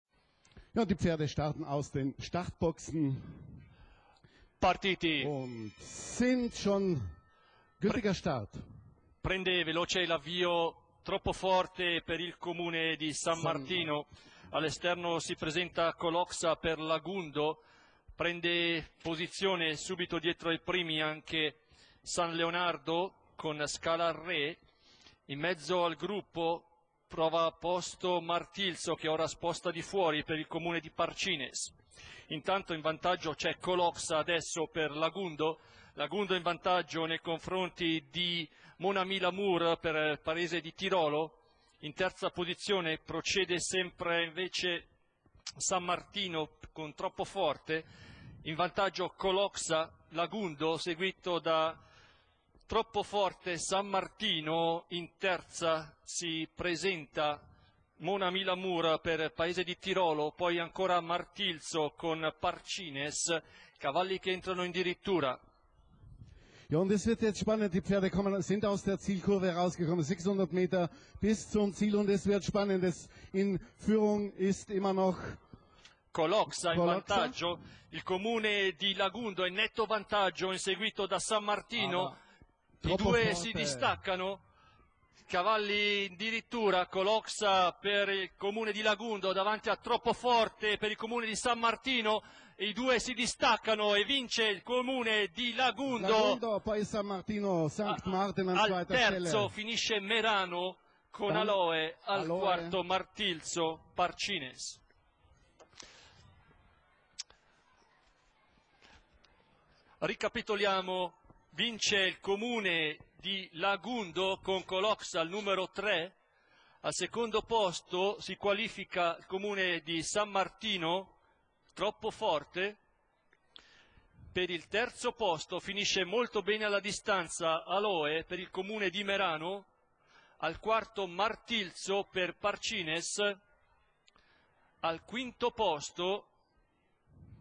22/09/2013, Merano , Premio San Genesio
1. COLOXA (MESSMER D.), 2. TROPPO FORTE (WENDEL CLAUDIA), 3. ALOE (DELLA PIETRA M.), 4. MARTILZO (PEDERSOLI GRAZIANO), 5. BOSCO CILIEGI (PRIKASKA J.), 99. SCALA AL RE (LUPINACCI A.), 99. CALA REGINA (URRU PAOLO), 99. MON AMI L'AMOUR (DE BORTOLI A), 99. SUPERFIAMMA (STEINWANDTER D.)